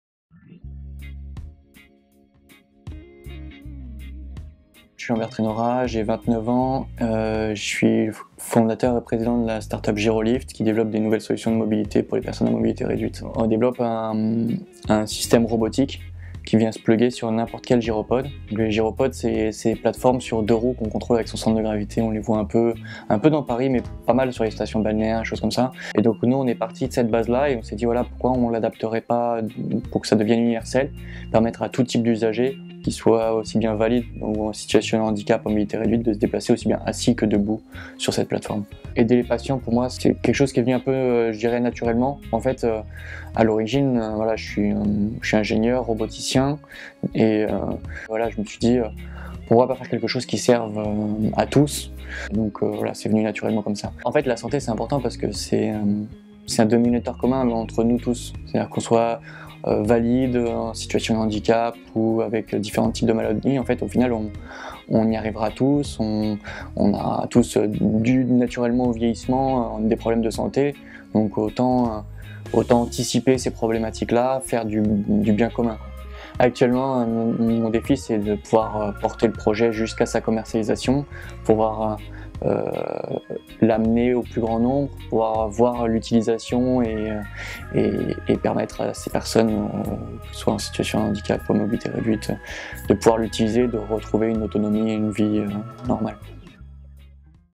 [MOBILISATION] Lambert a décidé de venir en aide aux patients
Lambert est le fondateur de Gyrolift, une start up qui accompagne les personnes en situation de handicap et les personnes à mobilité réduite. Grâce à un dispositif de mobilité basé sur un gyropode, cette innovation permet à son utilisateur de se déplacer aussi bien assis que debout. La maniabilité du gyropode alliée à la possibilité de se verticaliser améliore l'accessibilité.